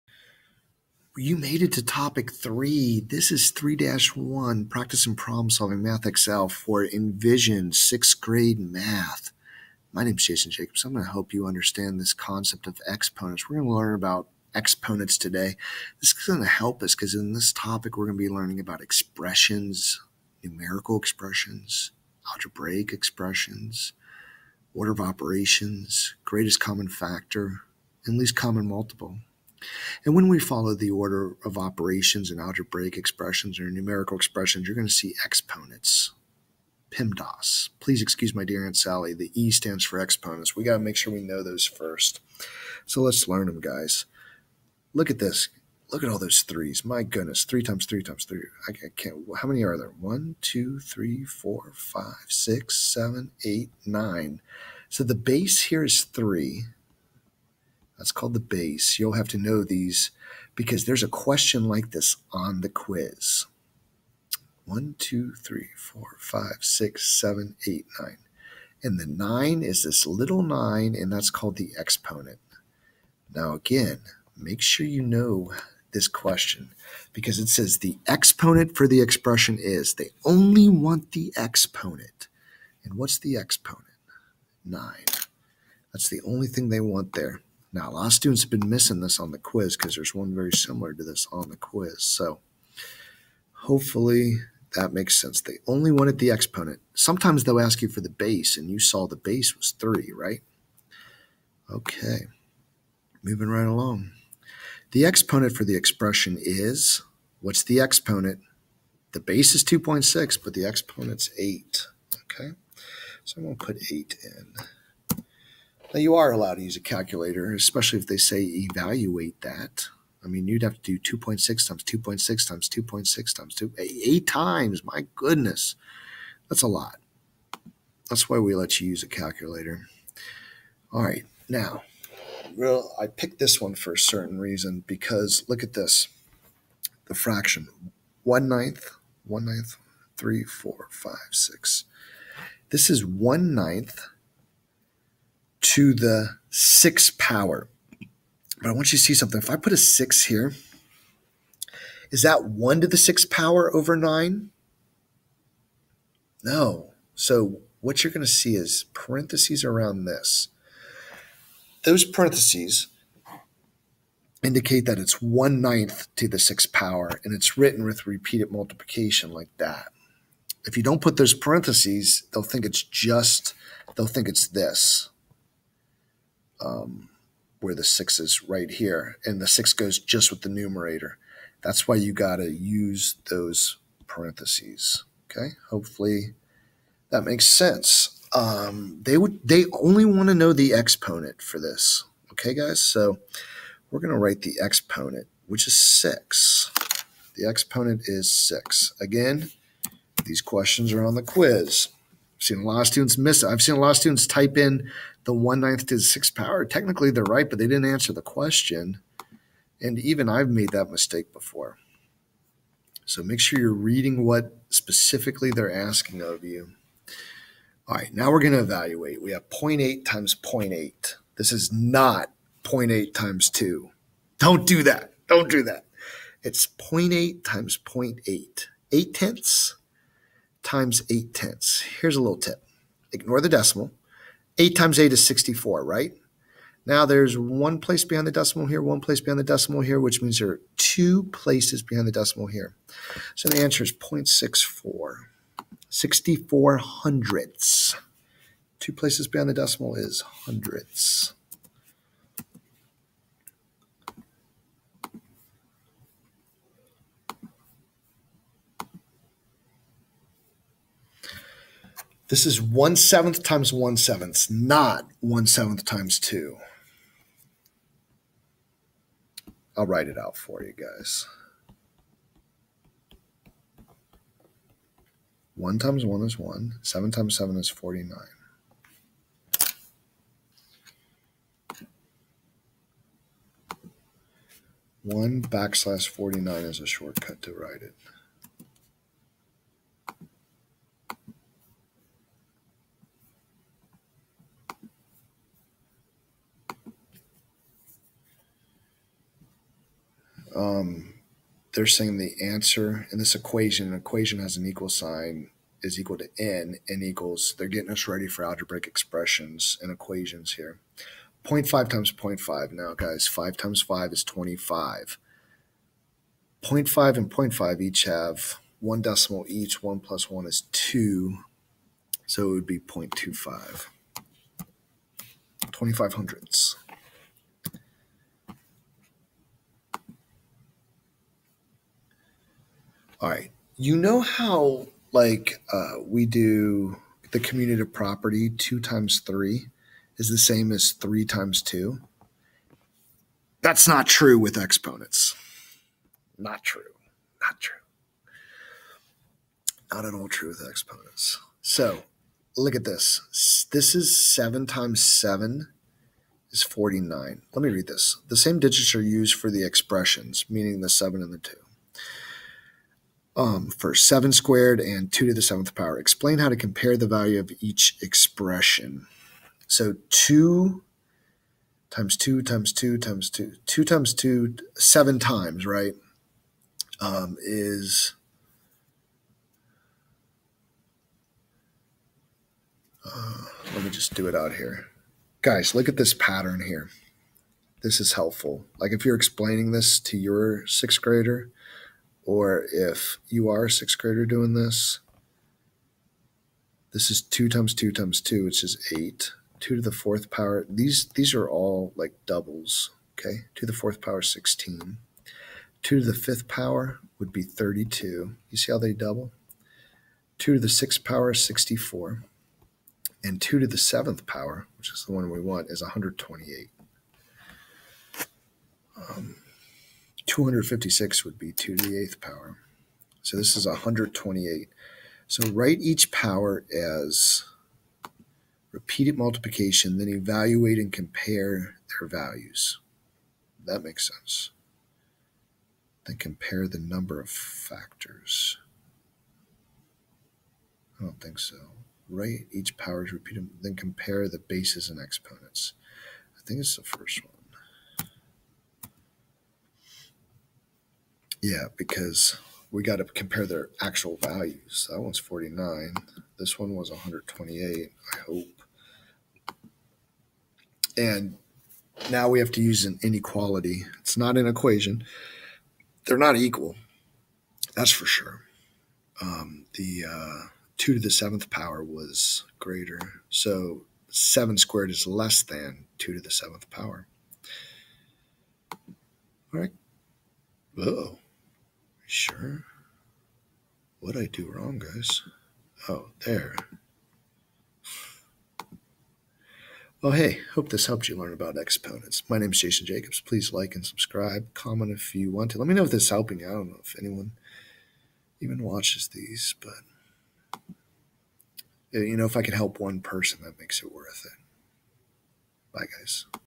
Exponents 3-1 Practice & Problem Solving Envision 2.0 Grade 6 Math
Exponents 3-1 Practice & Problem Solving Envision 2.0 Grade 6 Math for Pearson Realize.
Exponents Sixth Grade Math Lesson with Jason Jacobs.